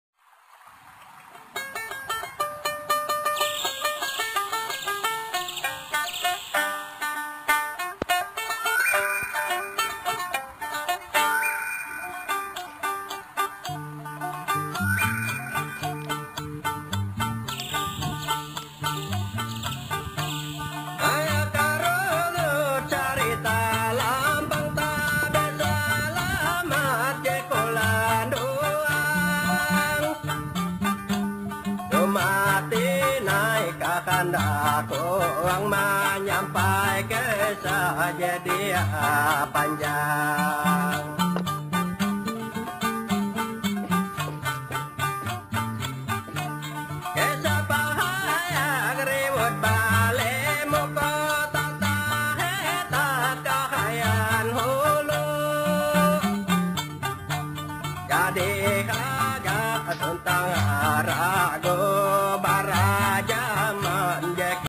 mandau tampuser 4 milik guru kuntau
Bilah/isi sengaja tidak diperlihatkan, bilah yg di vidio hanya pemanis, bukan bilah aslinya